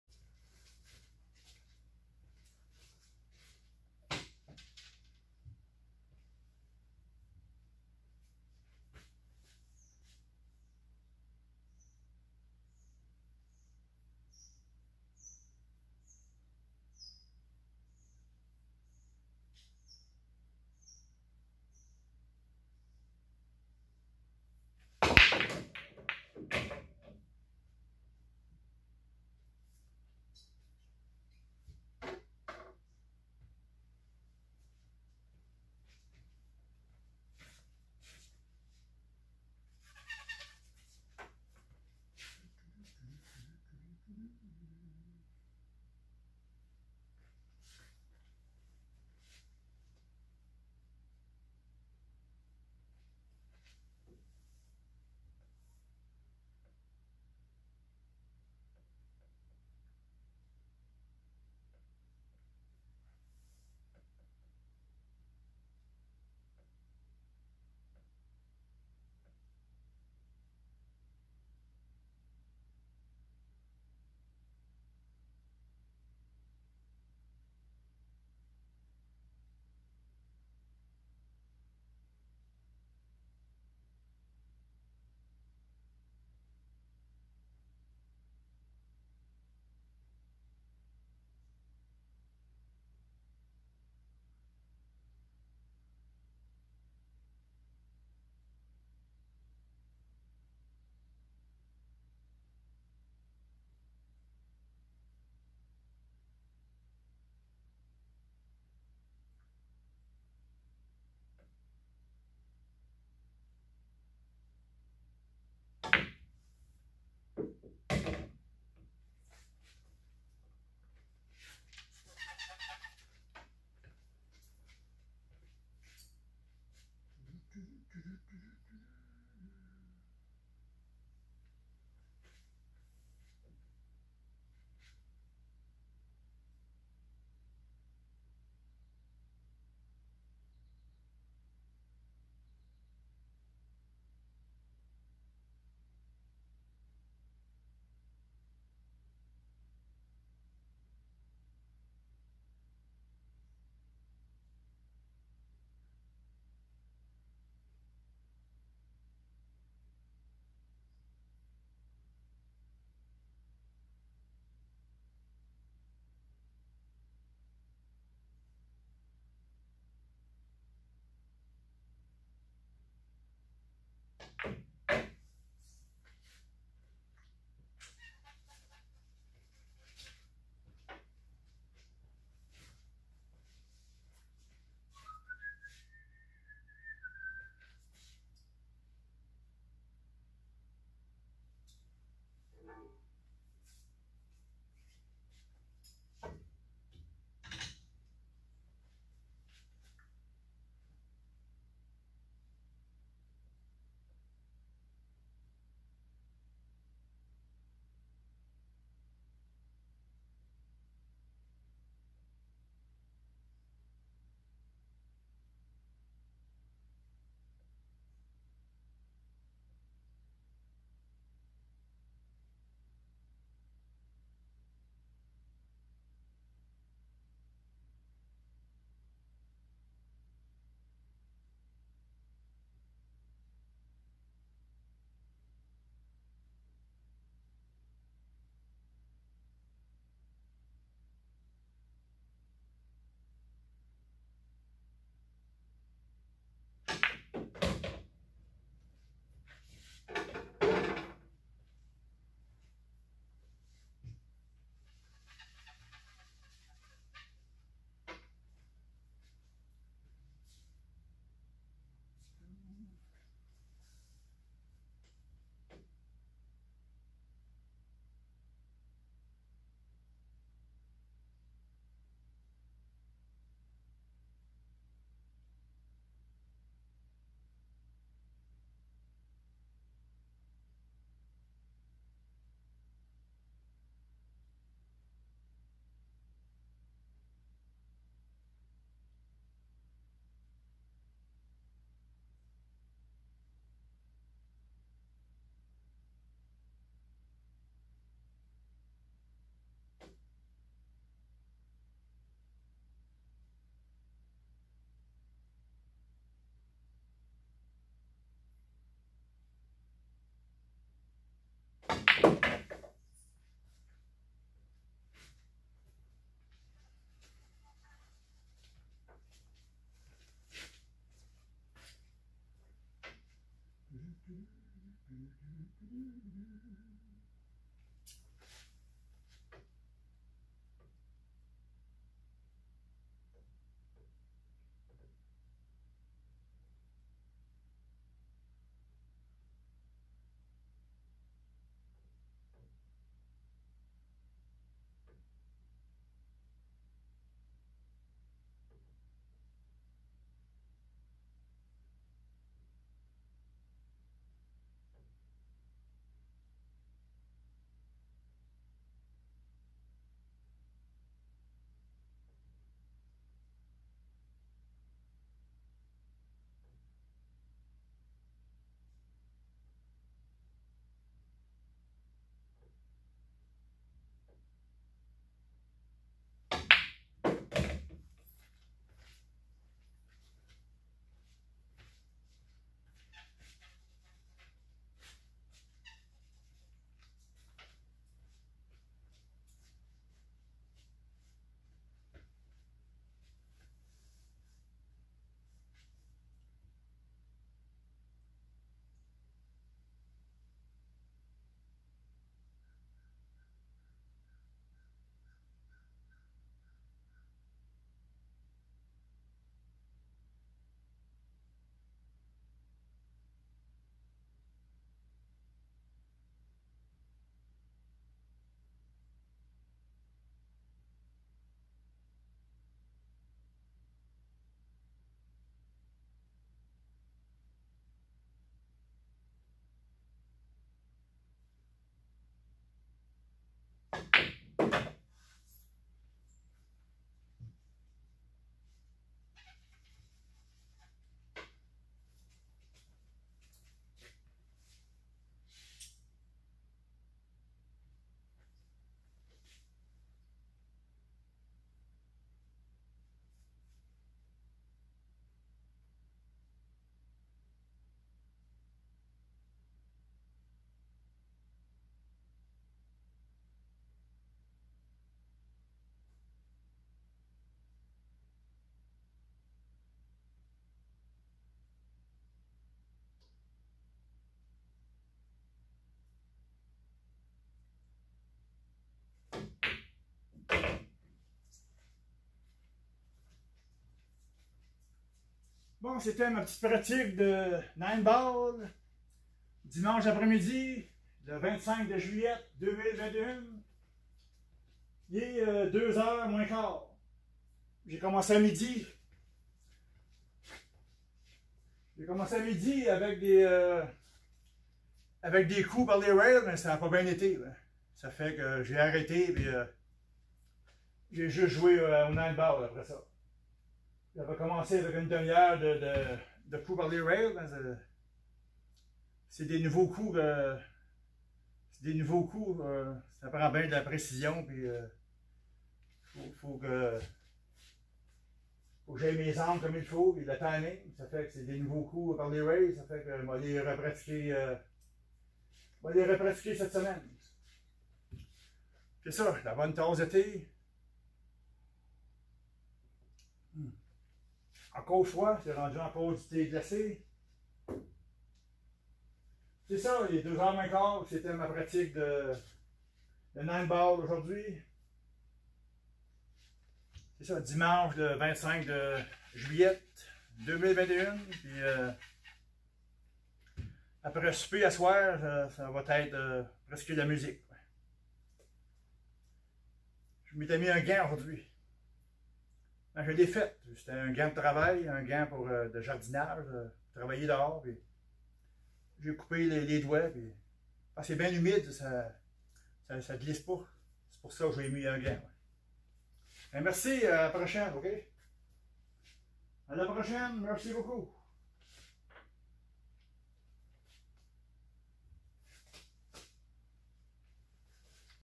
9 ballpractice-Sunday After noonJuly 25-2021-ChelseaQc
Sunday  After  noon  Nine Ball Practice  -- Mario Prud'Homme  -- 25 Juillet  2021- Chelsea Québec